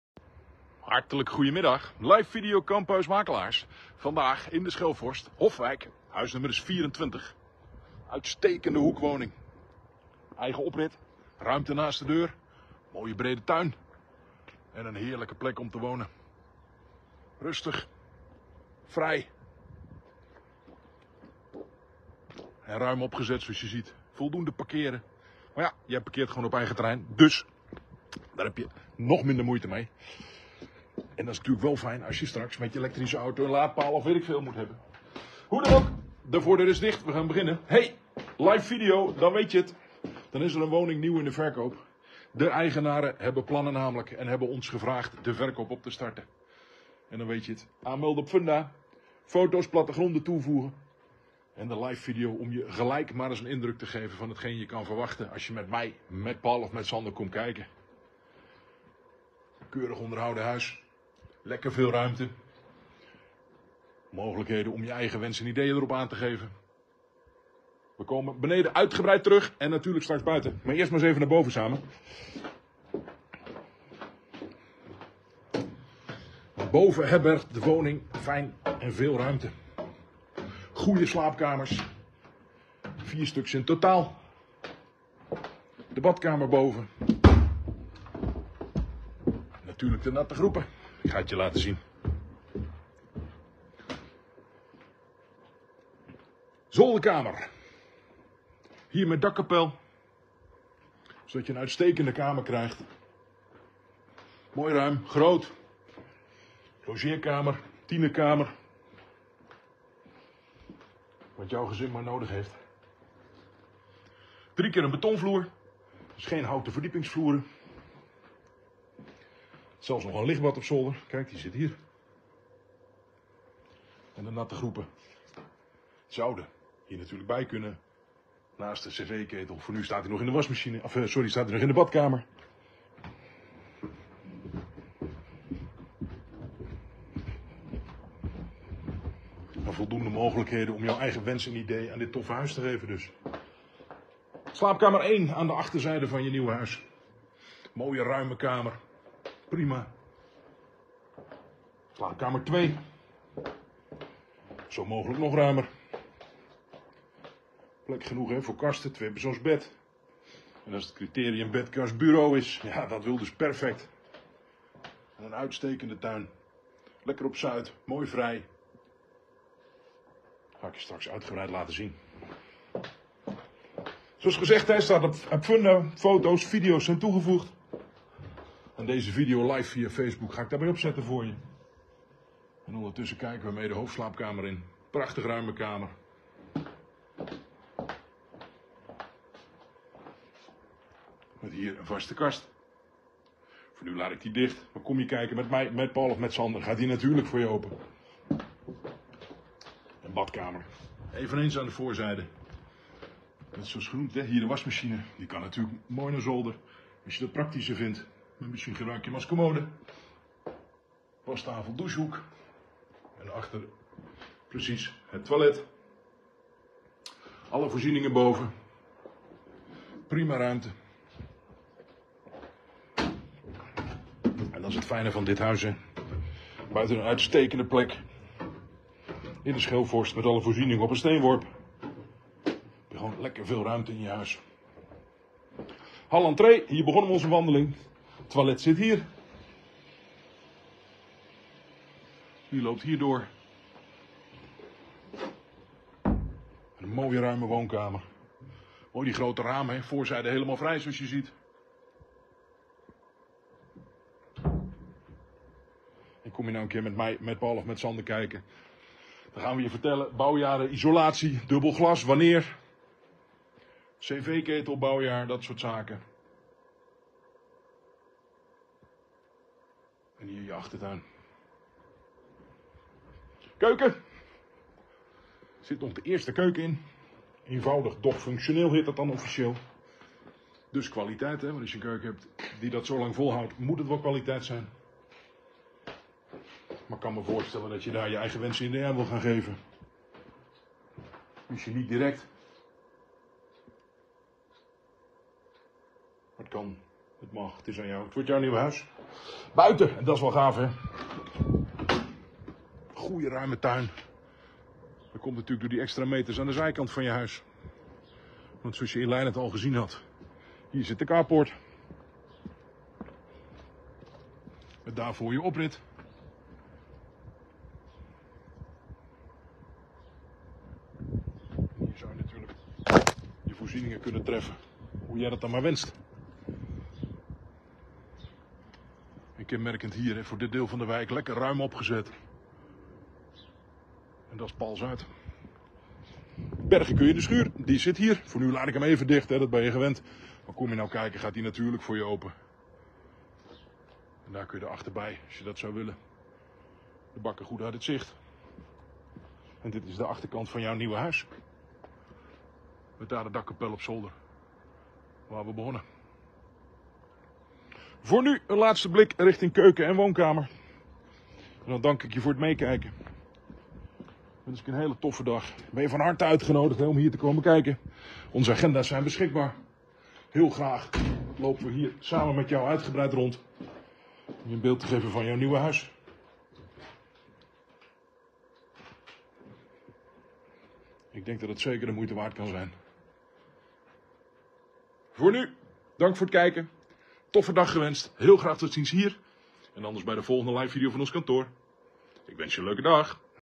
Live video Hofwijck 24, Almelo
"HARTELIJK GOEDEMIDDAG"👋😉

Jouw NR1 makelaar laat je graag kennis maken met;
Hofwijck 24, Almelo 🏠 🤩.

Met veel plezier nodigen we je o.a. om deze vijf redenen uit om met ons te komen kijken;
1. Fijne hoekwoning
2. Mooie vrije tuin op zuid
3. Lekker veel ruimte
4. 4 Toffe slaapkamers
5. Energie label D